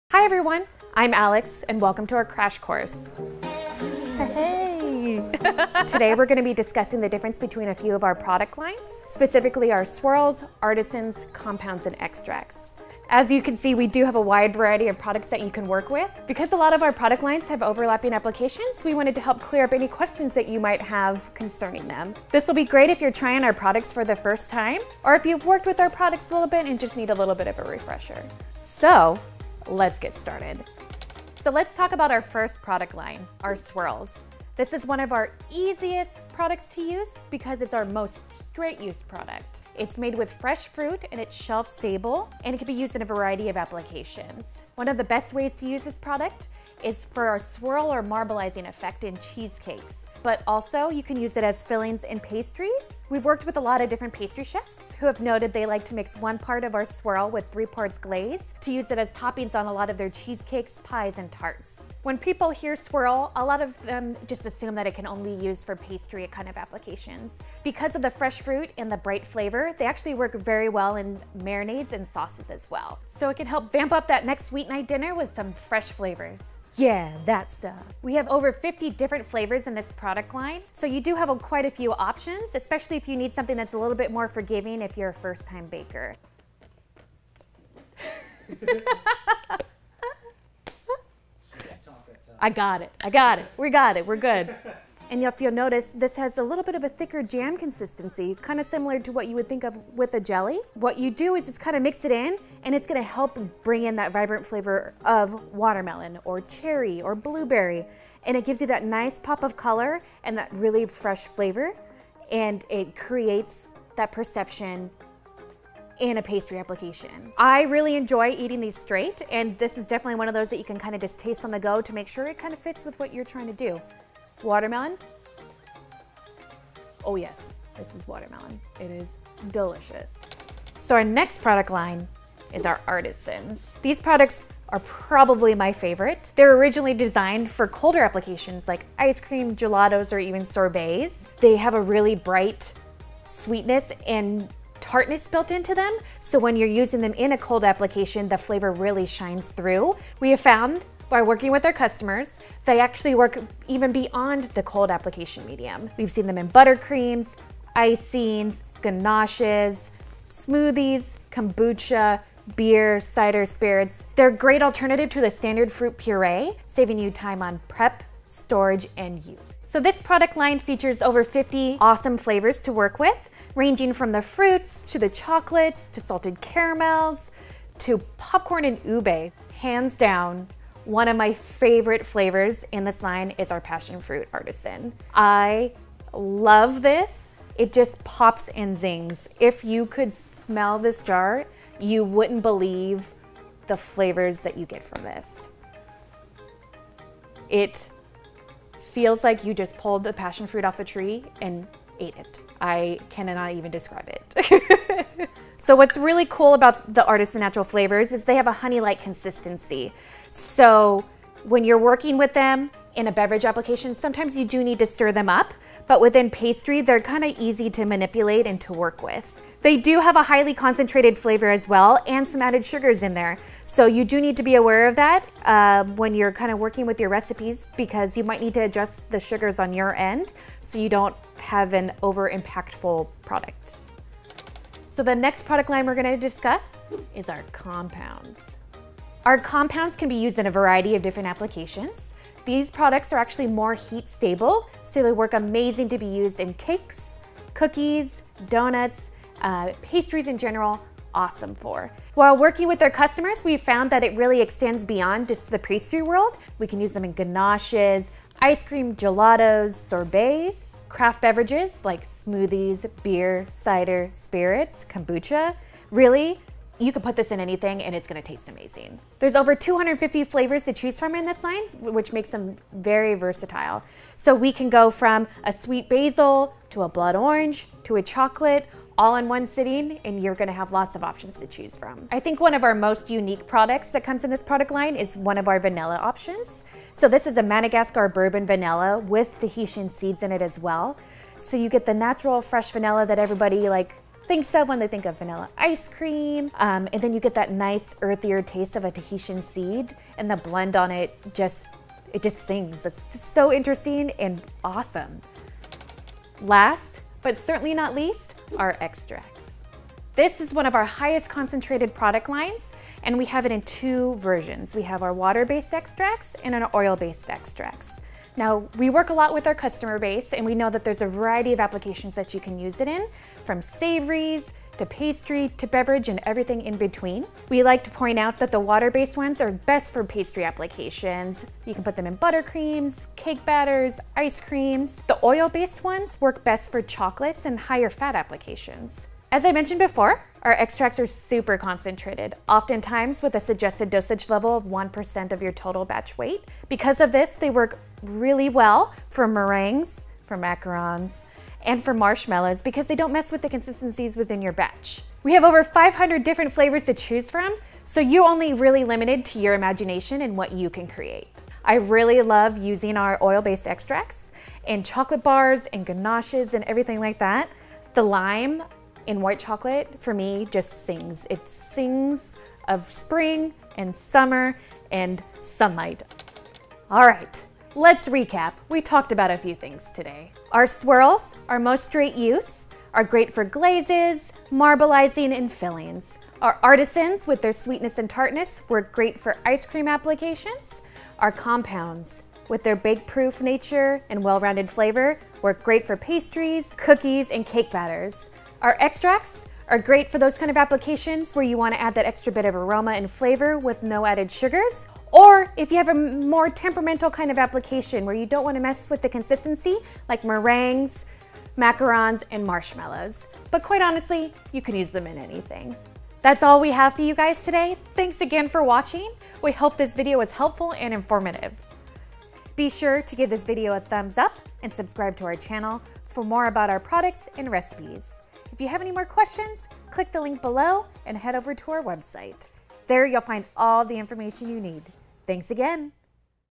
Amoretti Crash Course with Alex | Difference Between Swirls, Artisans, Compounds & Extracts 2022
While 7 minutes and 16 seconds is not quite enough time to cover all of the different categories, applications and use levels of Amoretti products it is enough time to give you a quick crash course on our most popular product lines.

In this video, we will cover the difference between our Swirls, Artisans, Compounds, and Extracts.